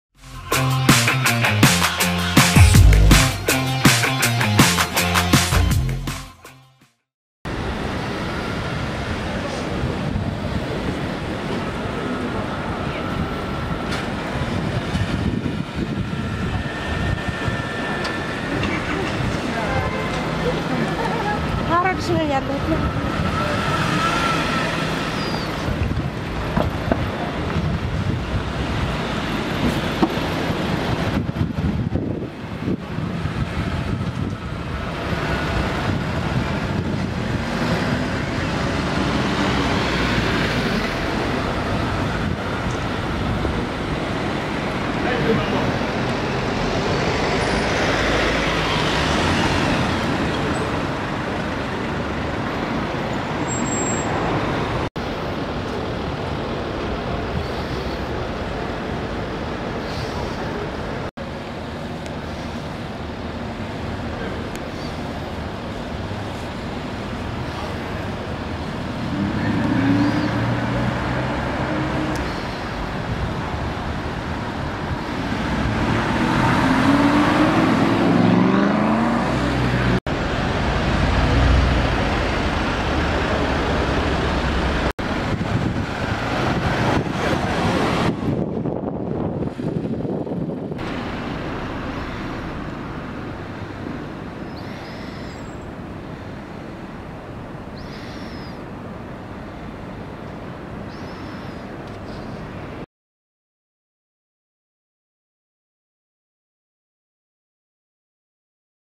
James Stunt & Petra Ecclestone Arrive at Harrods
When Businessman James Stunt and Petra Ecclestone go shopping they sure know how to arrive in style. The pair turned up in a Mansory Rolls Royce and a standard Phantom and a Mansory Range Rover.